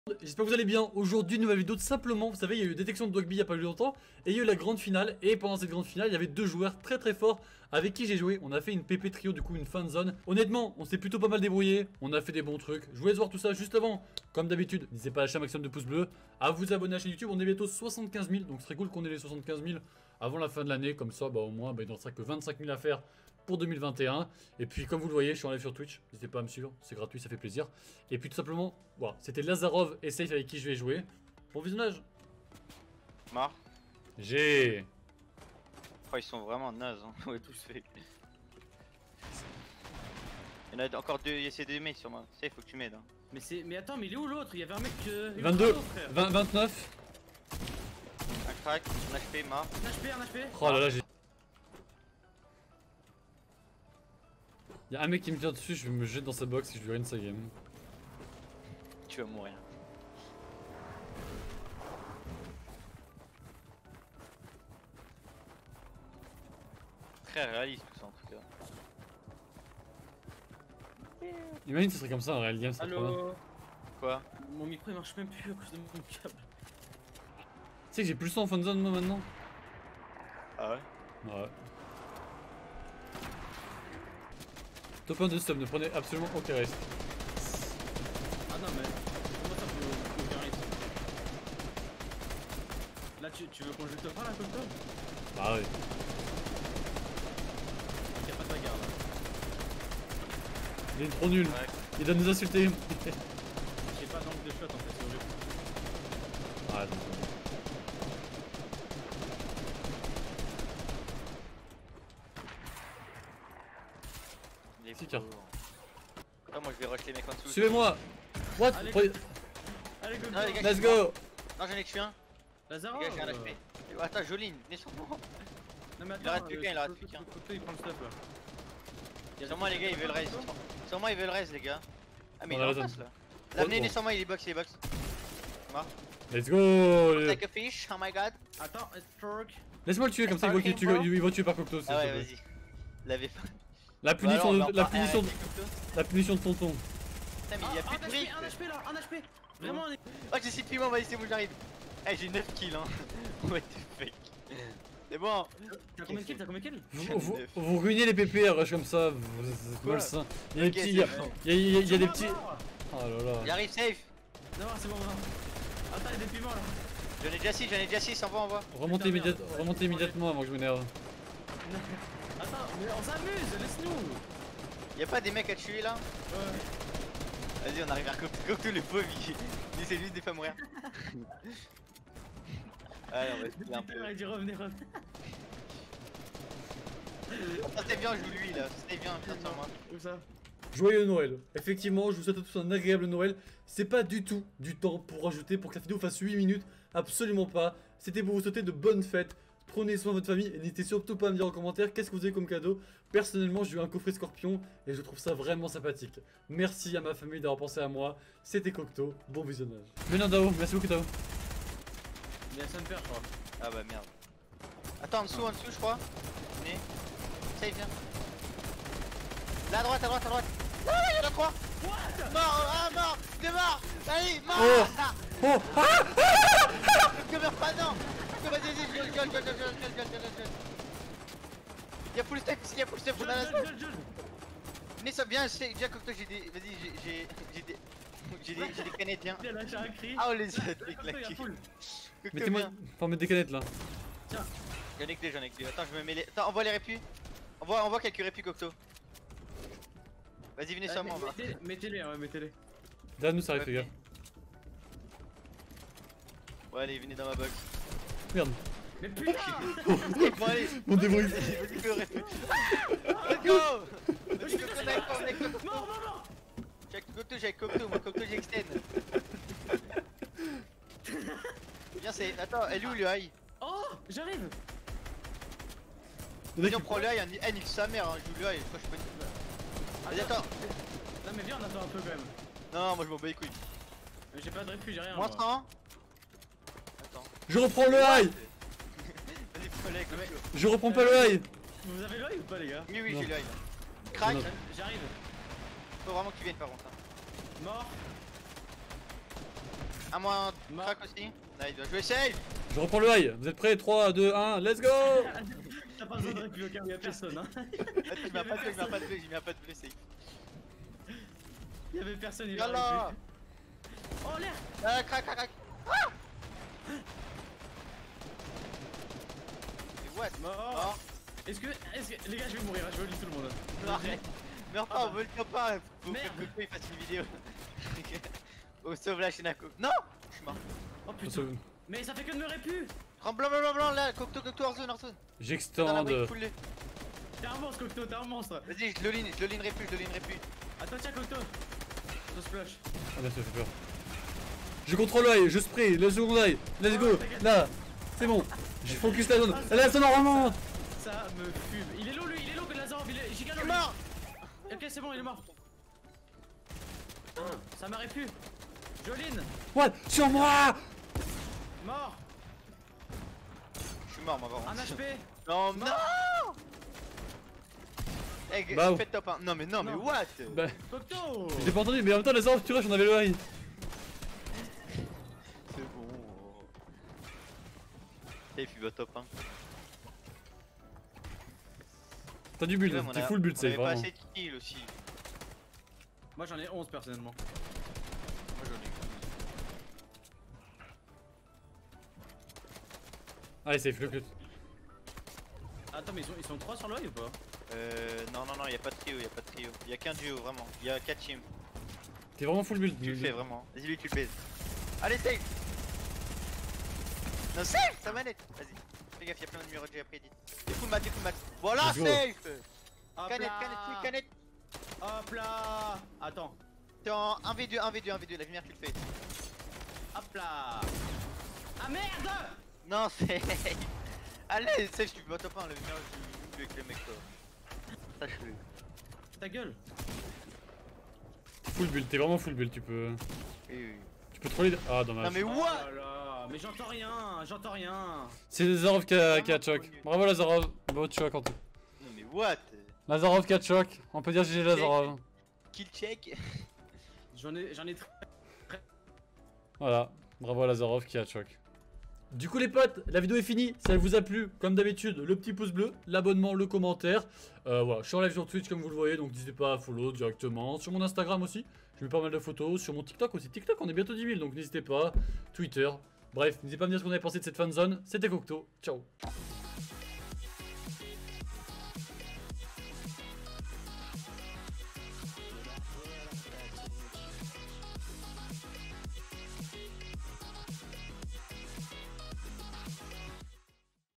JE TRIO AVEC DEUX FINALISTES DE LA DETECTION (Game avec storm surge)
Je joue avec des finaliste de la detection ils sont trop fort